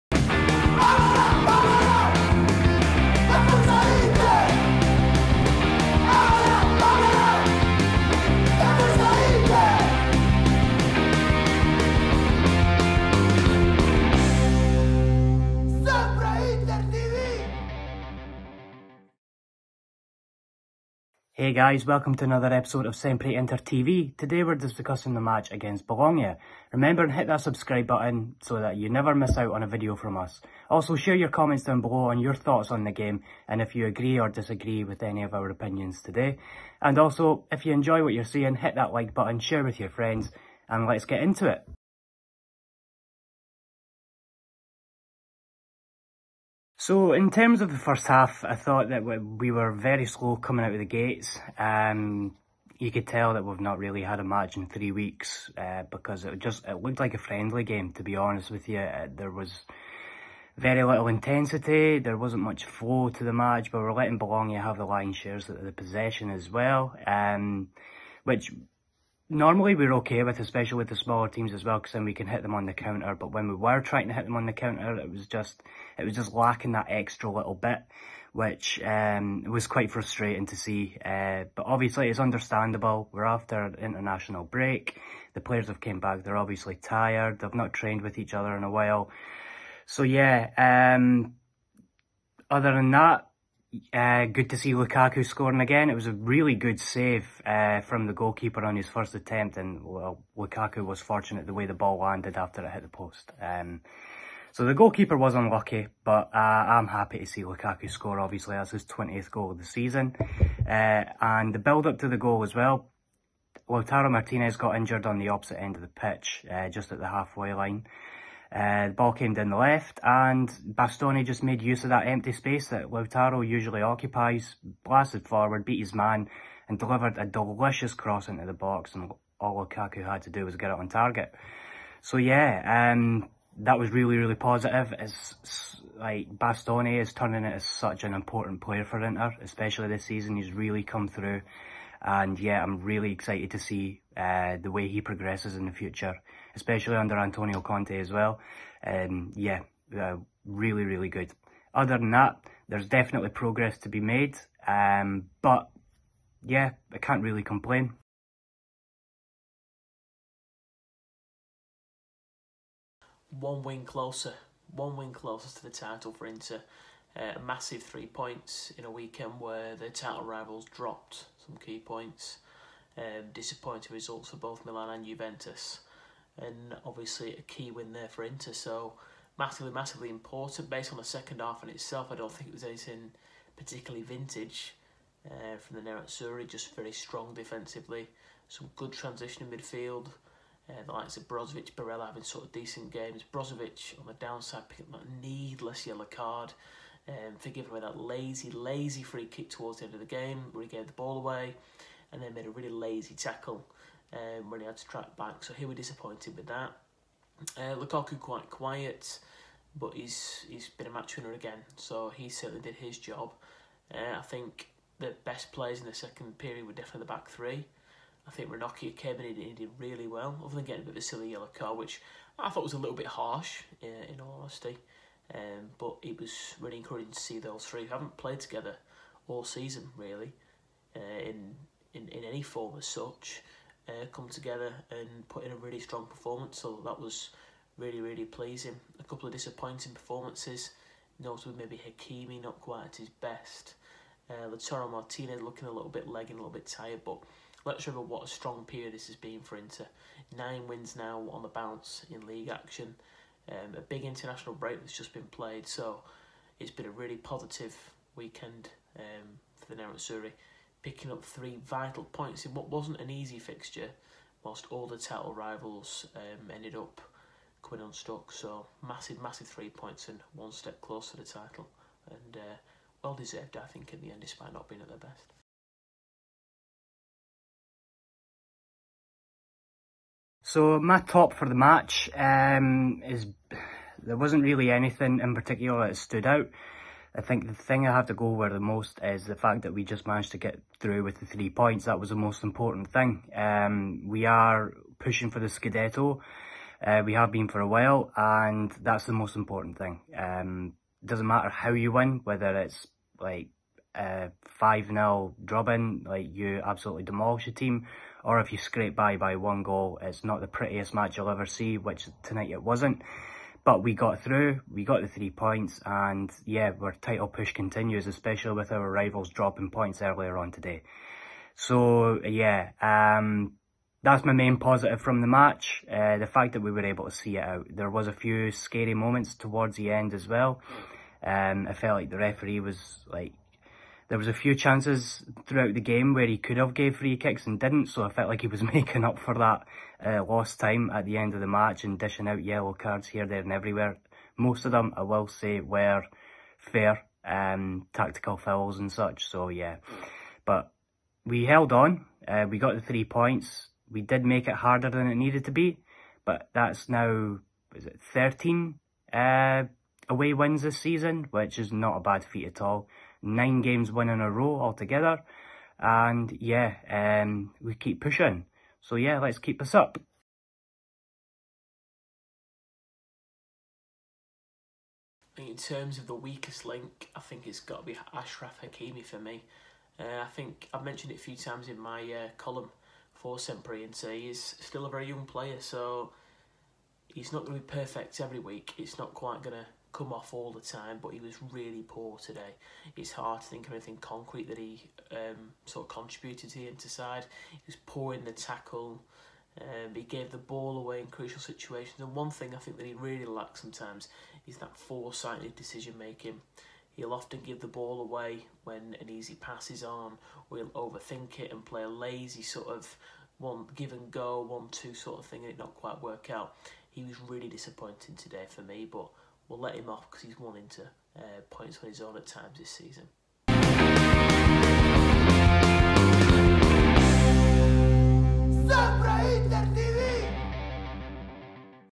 Reaction I Bologna 0 - 1 Inter I 9th Serie A Win On The Trot
In the latest SempreInterTV video, hosts Michael Findlay and Jake Smalley reacts to Inter’s  win away to Bologna on Saturday evening.

Romelu Lukaku put Inter ahead midway through the first half latching on to a great cross from Alessandro Bastoni to break the deadlock which turned out to be the only goal of the game.

Topics include:

- Alessandro Bastoni's delicious cross
- Romelu Lukaku scores 20th Serie A goal
- Scudetto number 19 very close now

Who was the best Inter player for you?